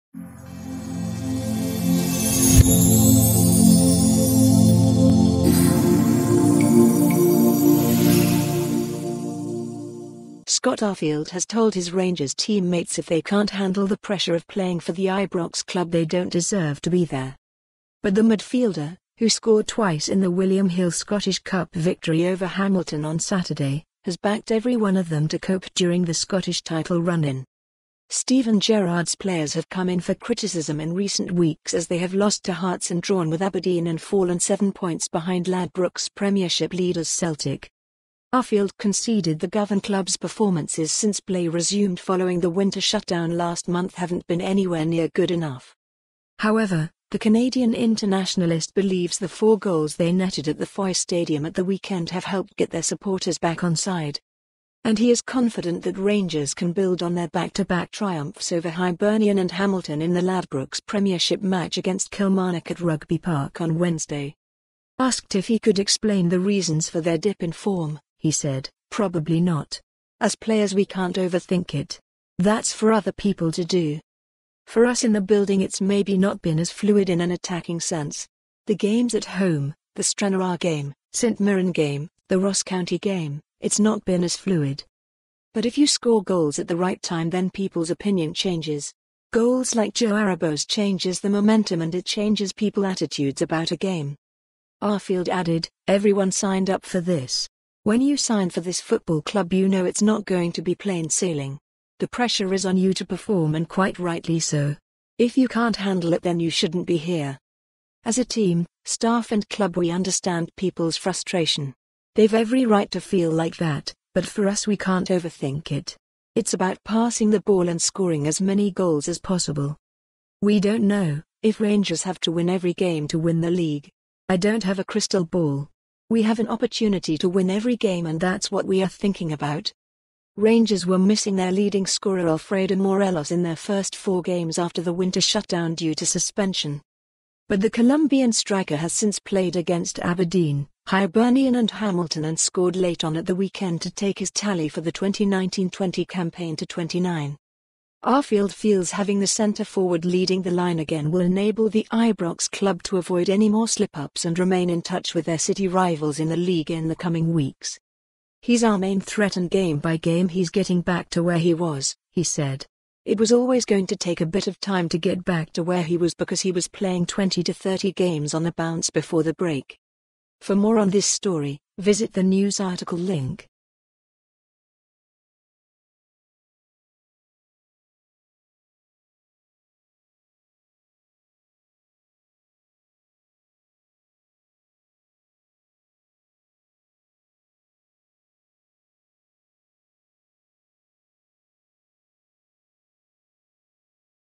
Scott Arfield: Players who can't cope with pressure shouldn't be at Rangers
SCOTT Arfield has told his Rangers team mates if they can't handle the pressure of playing for the Ibrox club they don't deserve to be there.

But the midfielder, who scored twice in the William Hill Scottish Cup victory over Hamilton on Saturday, has backed every one of them to cope during the Scottish title run-in.

Steven Gerrard's players have come in for criticism in recent weeks as they have lost to Hearts and drawn with Aberdeen and fallen seven points behind Ladbrokes Premiership leaders Celtic.

Arfield conceded the Govan club's performances since play resumed following the winter shutdown last month haven't been anywhere near good enough.

However, the Canadian internationalist believes the four goals they netted at the FOY Stadium at the weekend have helped get their supporters back onside.

And he is confident that Rangers can build on their back-to-back triumphs over Hibernian and Hamilton in the Ladbrokes Premiership match against Kilmarnock at Rugby Park on Wednesday.

Asked if he could explain the reasons for their dip in form, he said: "Probably not. As players we can't over think it. That's for other people to do.

"For us in the building it's maybe not been as fluid in an attacking sense. The games at home, the Stranraer game, St Mirren game, the Ross County game, it's not been as fluid.

"But if you score goals at the right time then people's opinion changes. Goals like Joe Aribo's changes the momentum and it changes people attitudes about a game."

Arfield added: "Everyone signed up for this. When you sign for this football club you know it's not going to be plain sailing. The pressure is on you to perform and quite rightly so. If you can't handle it then you shouldn't be here.

"As a team, staff and club